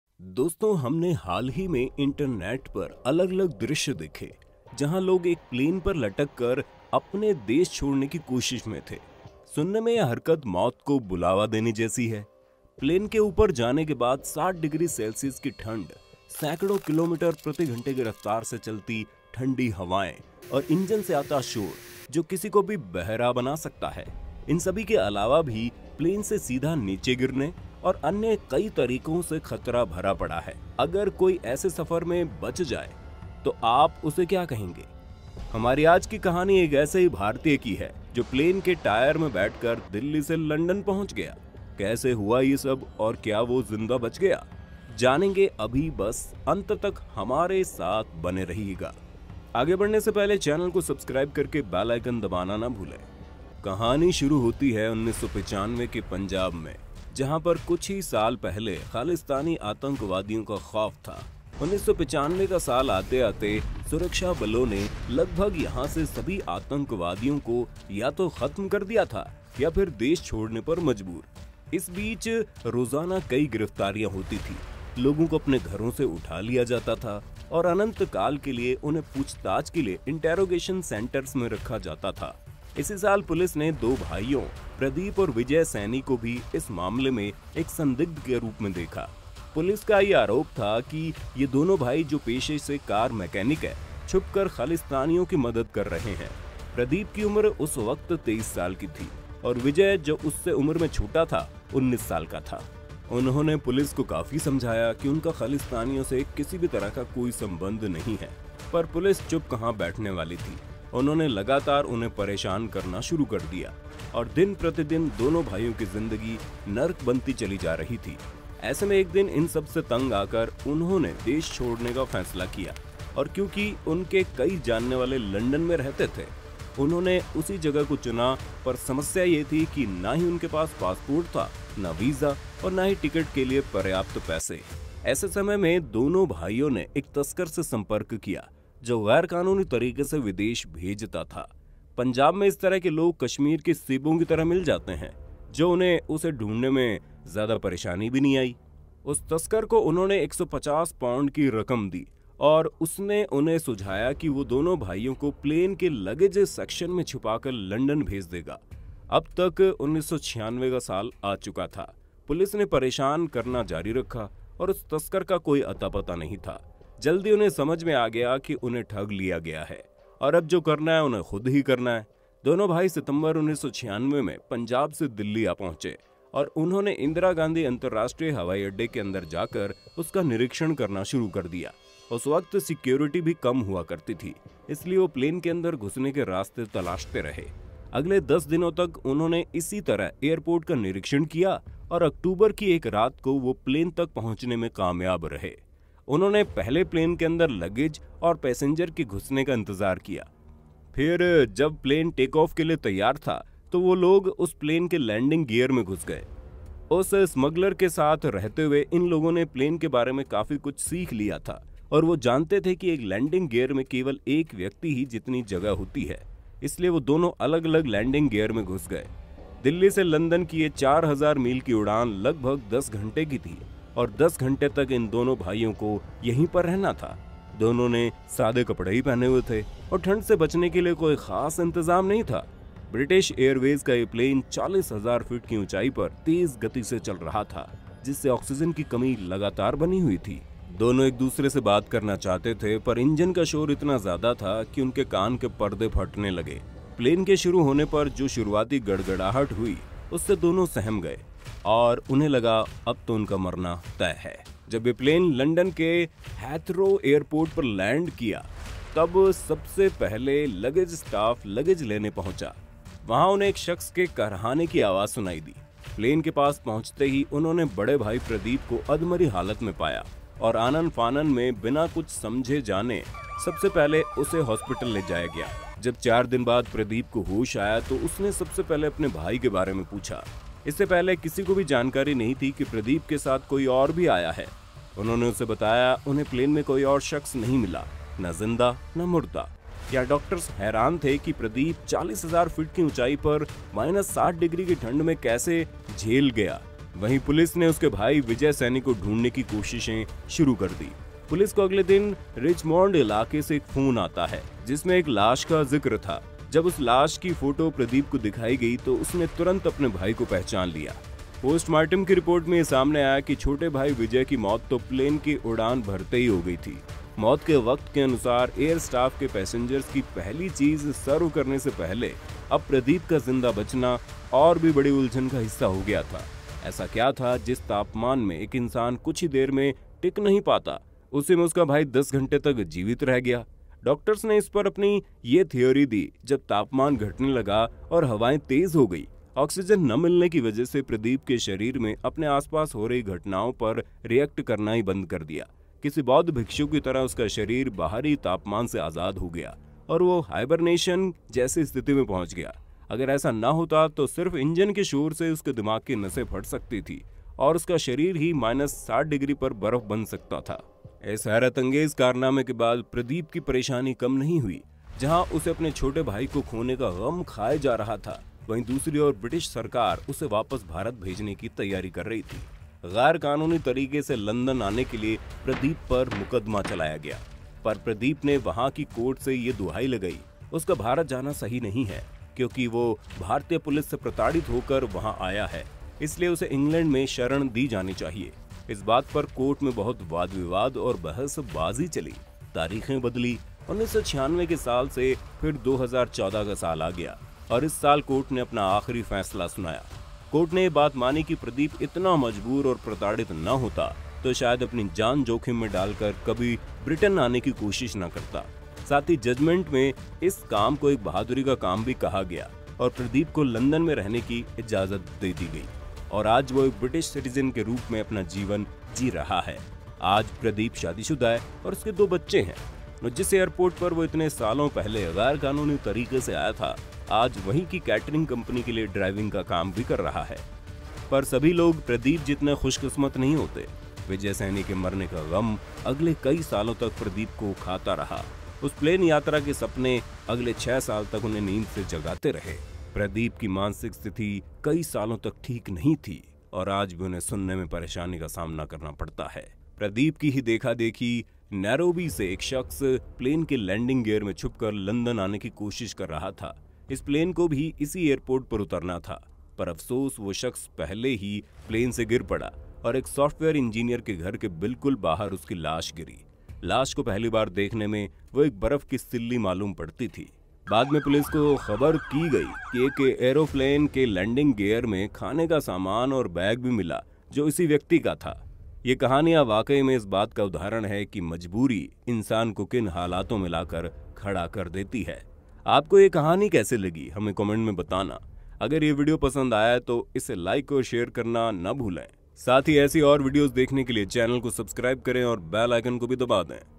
एयरप्लेन के टायर पर लटक कर भारत से भागे और फिर जो हुआ.. | Punjabi Sikh Saini Brother's Real Story
Video Title : एयरप्लेन के टायर पर बैठ कर भारत से भागे और फिर जो हुआ.. | Punjabi Sikh Saini Brother's Real Story
Language  : Hindi